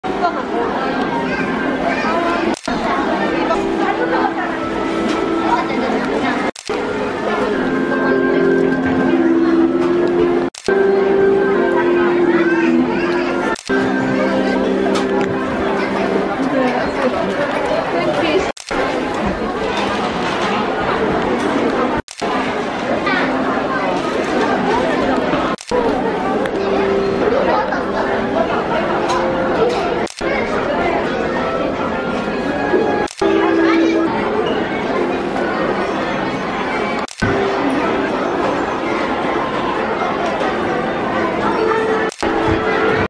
すみっコぐらし展初日 ～みんなでいっしょに星空演奏会～ 朝行列必至の大混雑でした 大丸神戸店 すみっこぐらし Sumikkogurashi
2022年1月5日 今日から開催　11時前に会場に着きましたが、
会場入り口の写真撮影も購入のレジも既に大行列でした
購入は諦めました
下のオモチャ売り場にも
少しすみっコグッズは売ってましたので、
オモチャ売り場での購入も良いかも知れません